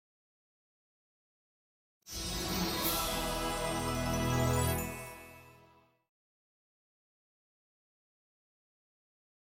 Walt Disney Home Entertainment (Blue background) Widescreen (Short audio)
Used on few NTSC Widescreen animated DVD releases from March 19, 2002-August 20, 2002.